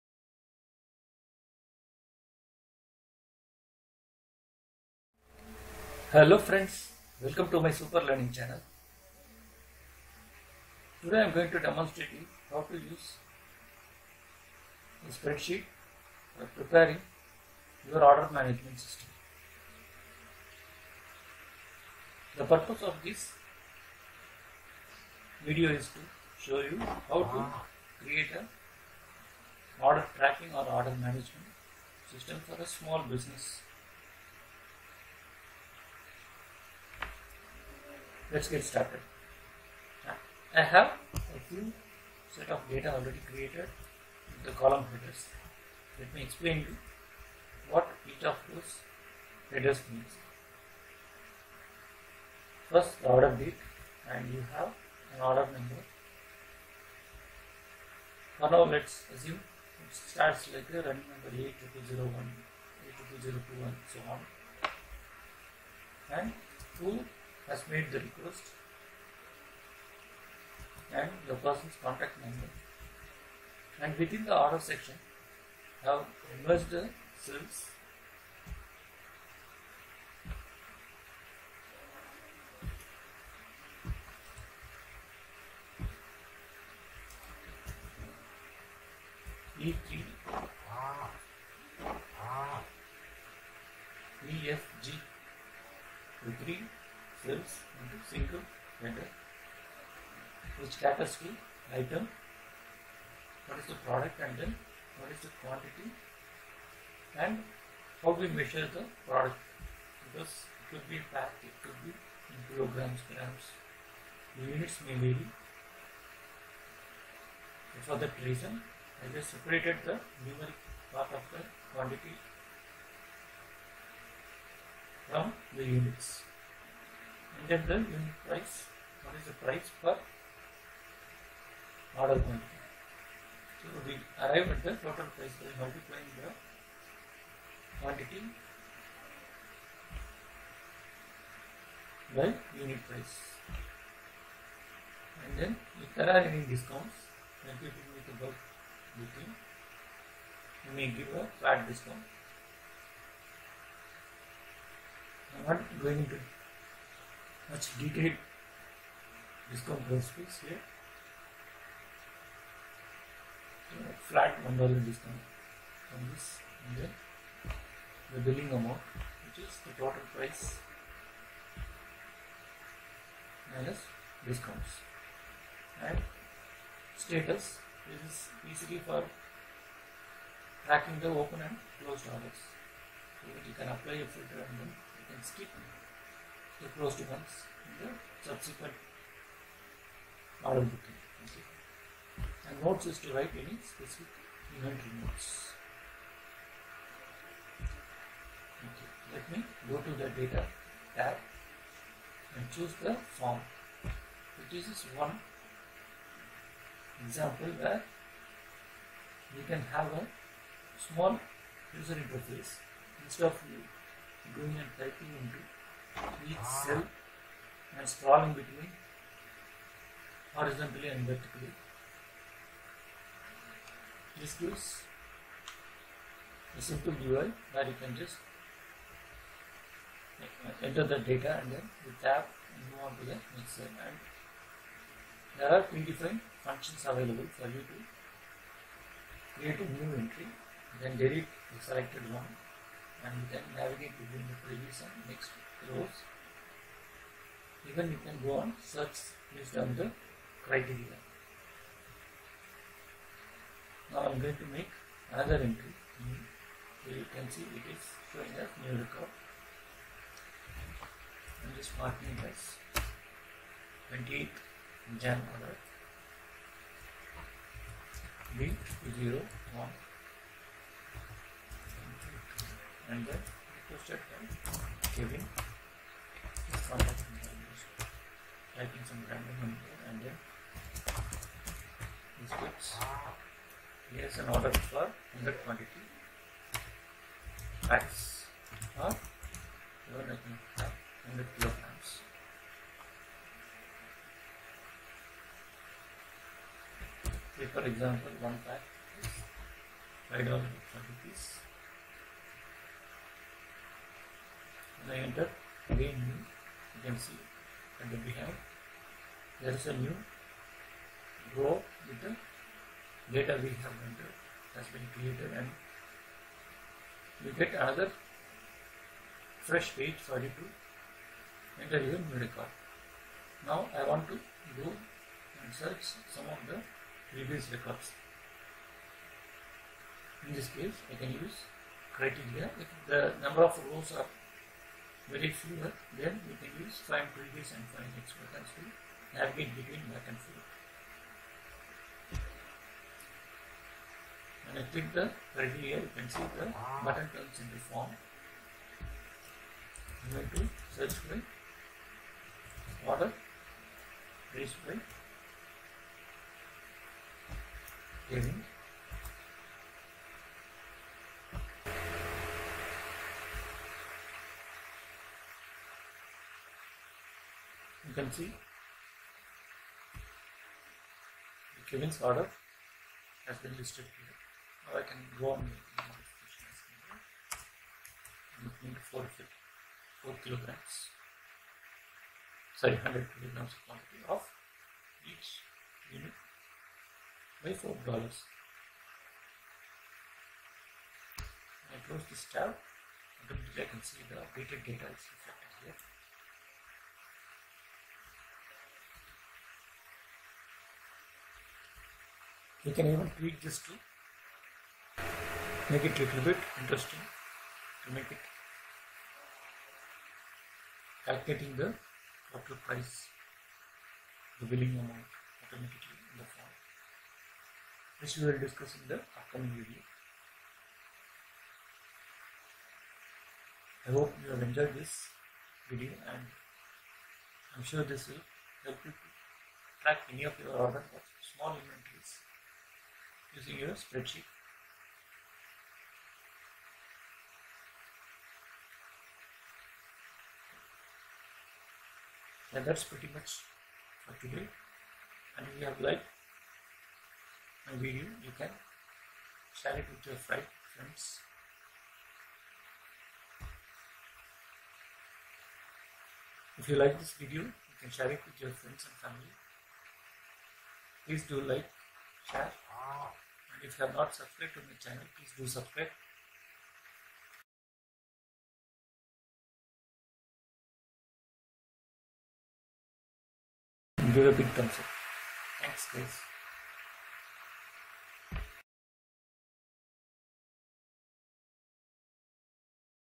Small Order Management using the spreadsheet.
This video has a simple template for preparing an order Management using the spreadsheet. This will come handy for those who want to track their small business inventory.